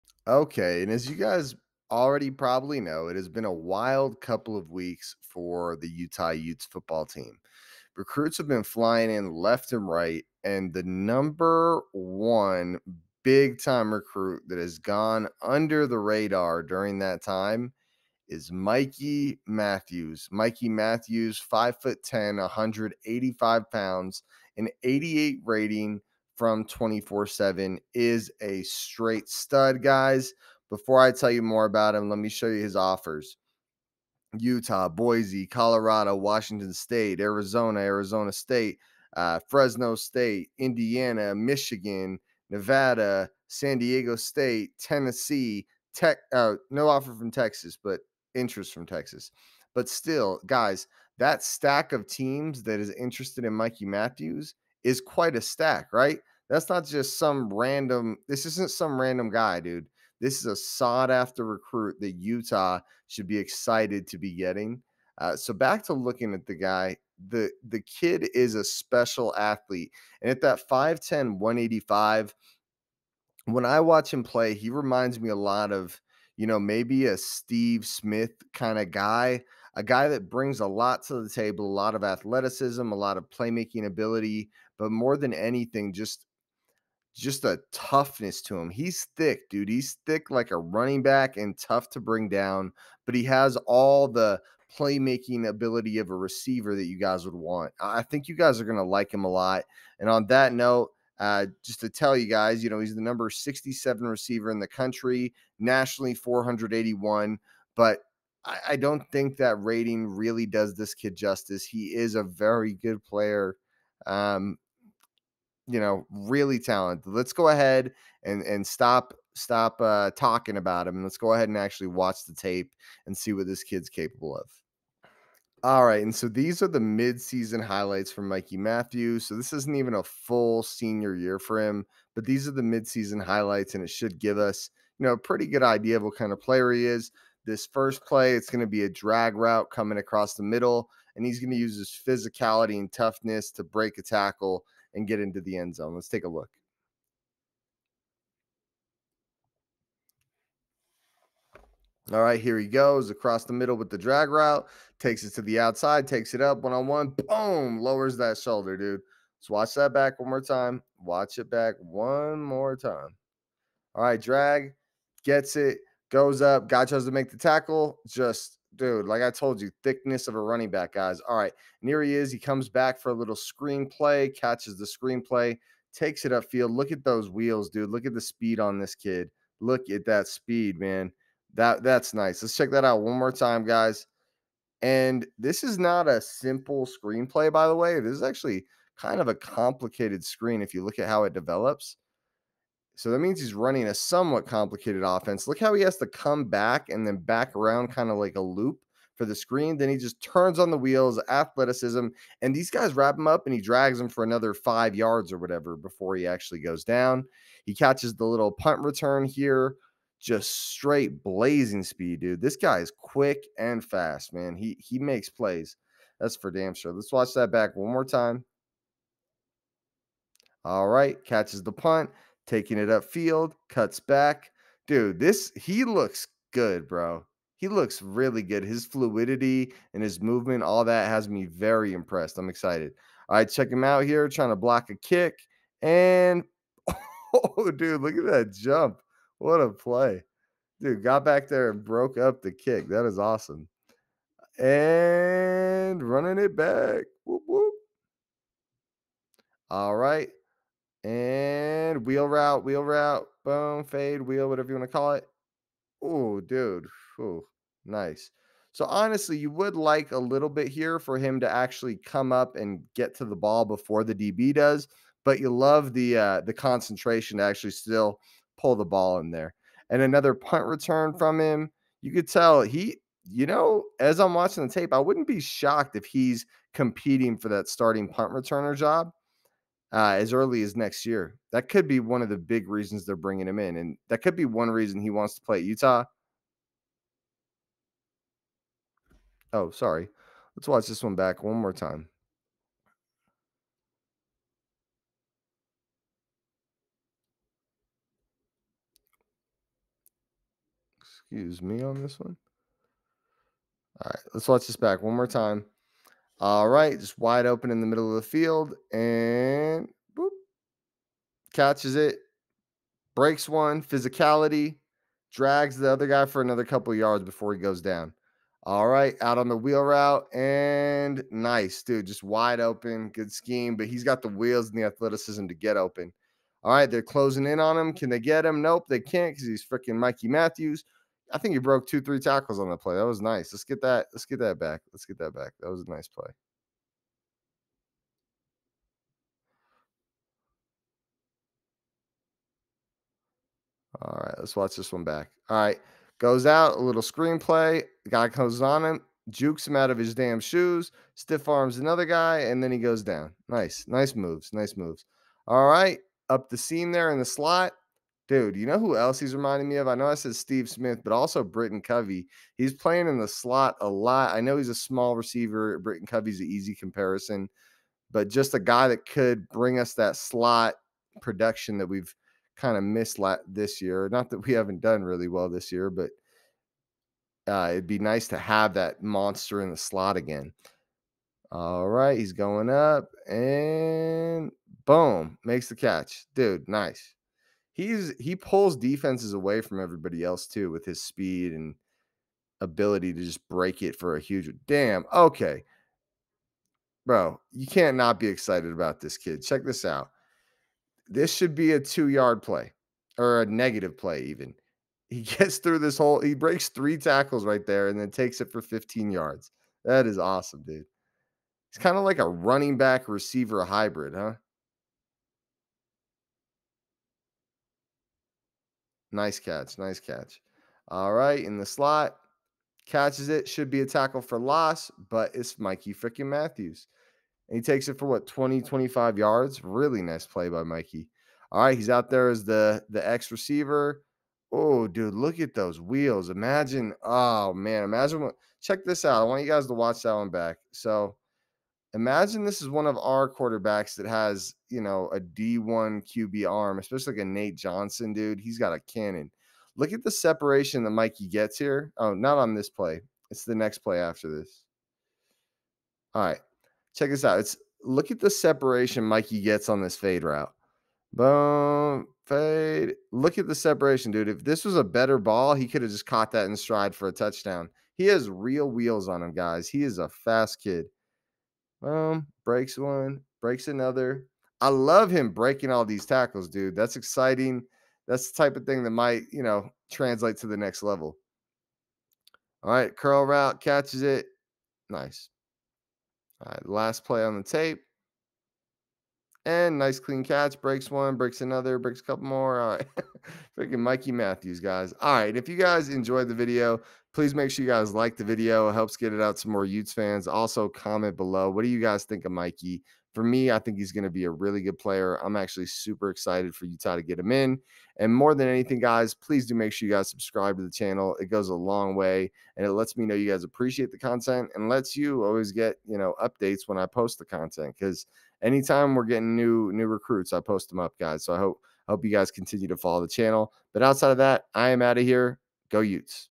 Mikey Matthews - Utah Utes Recruiting
Mikey Matthews went a little under the radar when he commited, but don't let that fool you into thinking he's not a big time player. This guy brings a playmaking ability in the screen game, can make plays out of the backfield and can go downfield to take the top of a defense.